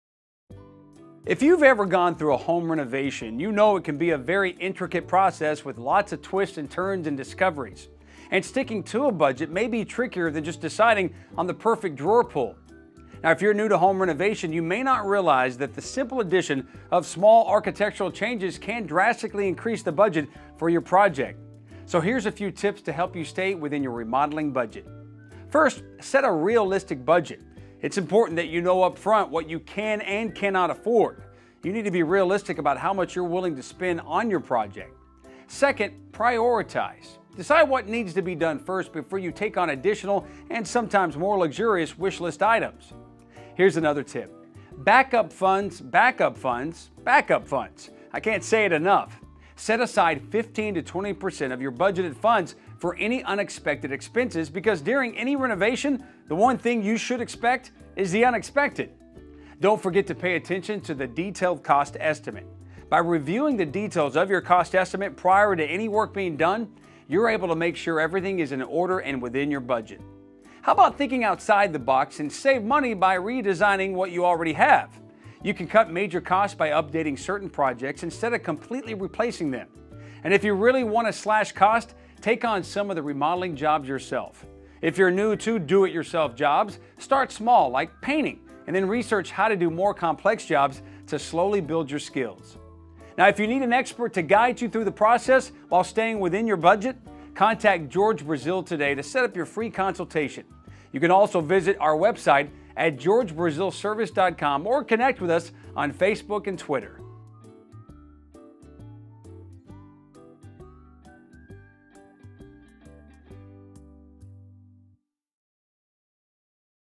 Remodeling A House On A Budget: Money Saving Tips
Remodeling a house on a budget is possible with these money saving tips. Let George Brazil help with your next home remodel.